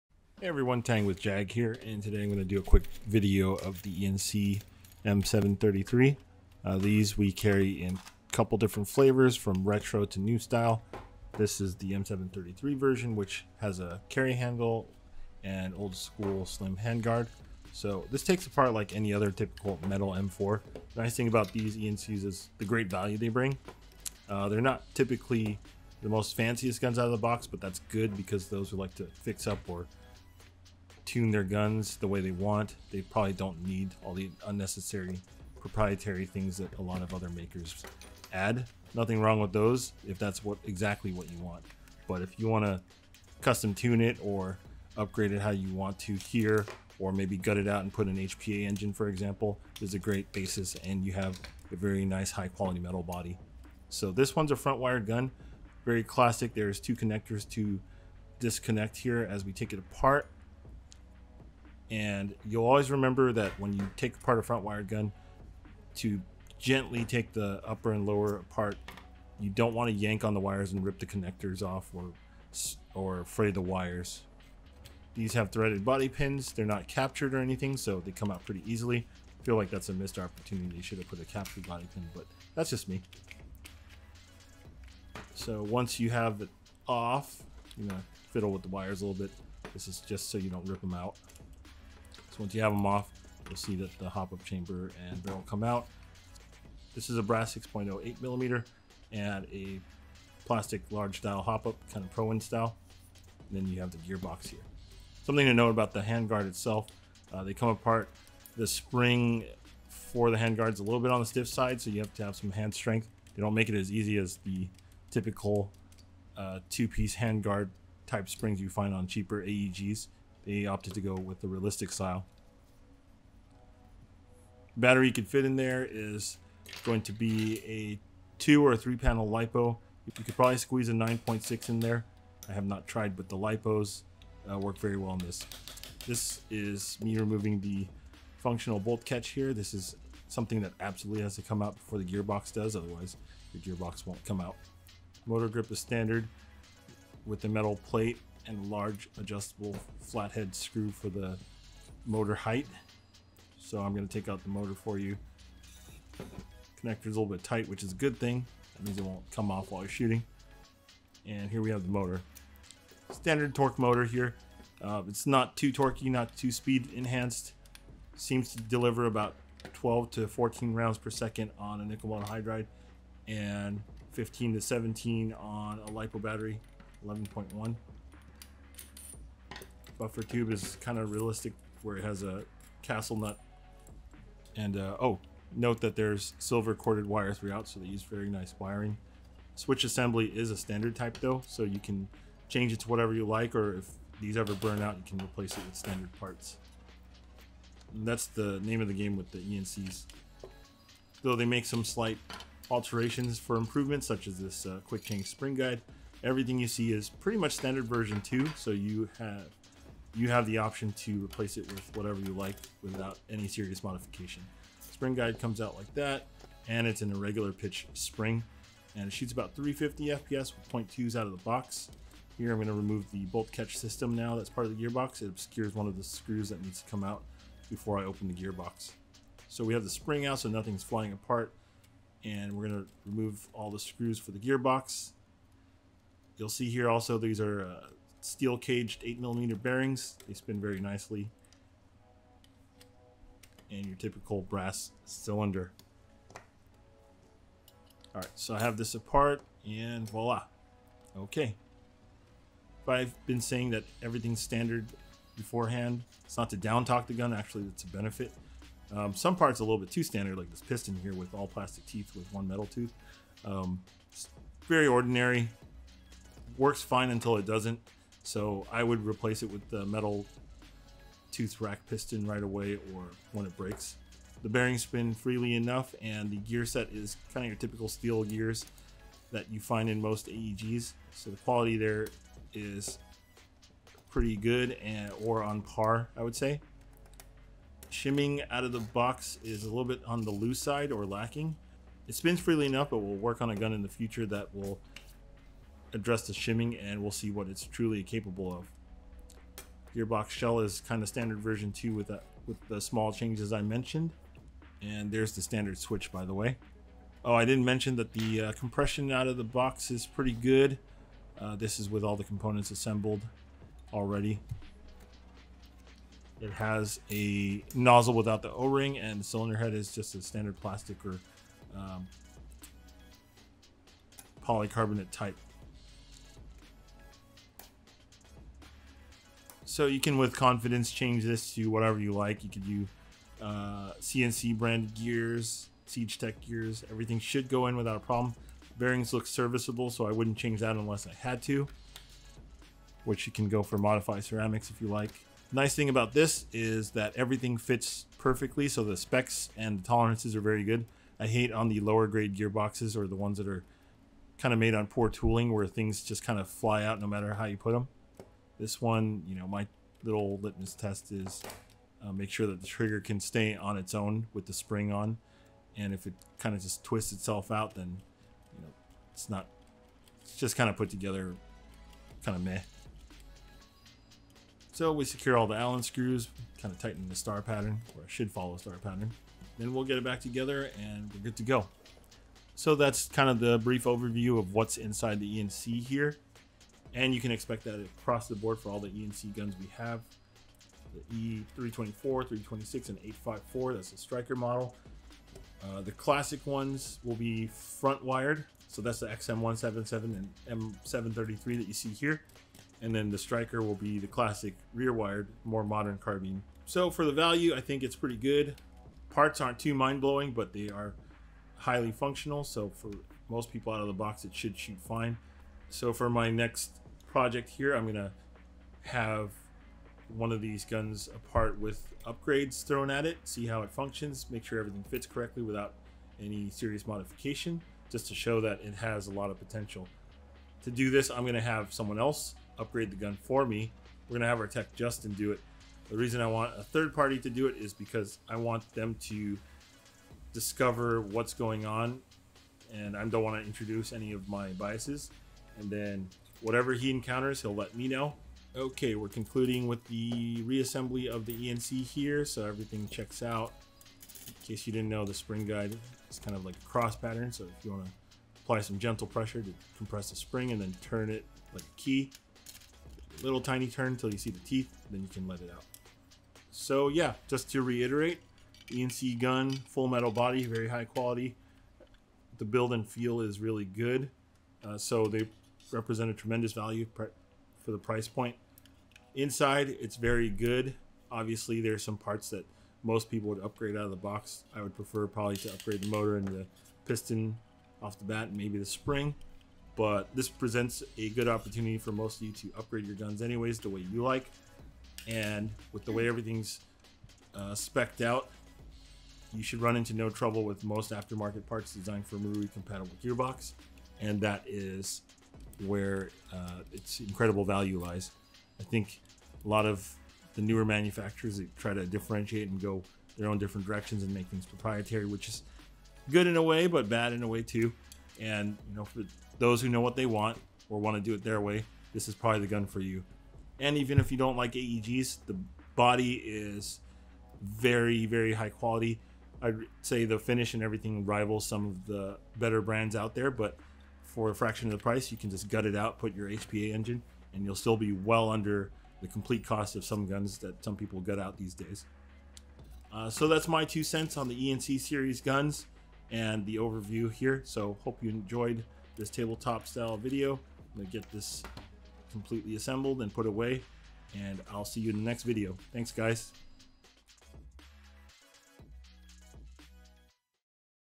Inside the Gearbox: E&C M733 (EC-326) - JAG Precision
We're taking a look at what's inside E&C's M733 or EC-326. These full metal AEGs are a fantastic value with very standardized components that make upgrading a hassle free affair. Great for those who want to build up their airsoft rifle the way they want in a really nice metal body. Available at JAG Precision dealers. If they don't have it, tell them this is what you want!

In the next video we'll throw some upgrades in to show how well it takes the parts.

E&C M733 (EC-326)

Features:
- full metal, available in retro style XM177, M16VN, and M733 or modern Striker CQB carbine
- 8mm gearbox
- quick change spring guide
- rotary hop up chamber
- functional bolt catch and release
- silver cord wiring

Support your local airsoft shops and airsoft fields!